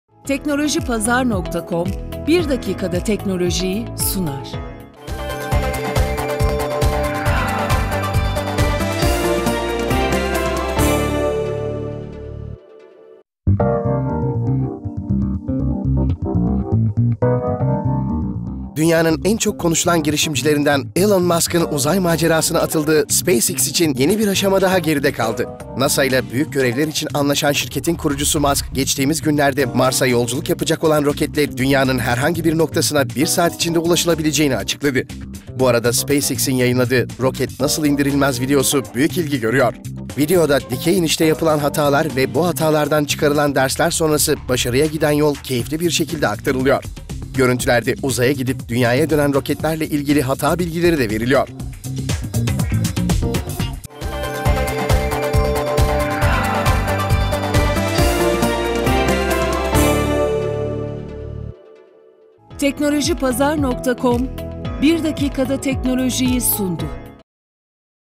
1 Dakika Teknoloji - SpaceX Roketlerin Macerası
Yapay zekanın dünyayı kuşatan, yakın bir tehlike olduğundan dem vuran Elon Musk, roketlerini dünya çapında yolcu taşımacılığında kullanmayı tasarlıyor. Bu yöndeki planını IAC'de yaptığı sunumla anlatan Musk, yolculukların yarım saat ila bir saat gibi kısa sürelerde tamamlanmasının sağlanacağını belirtiyor.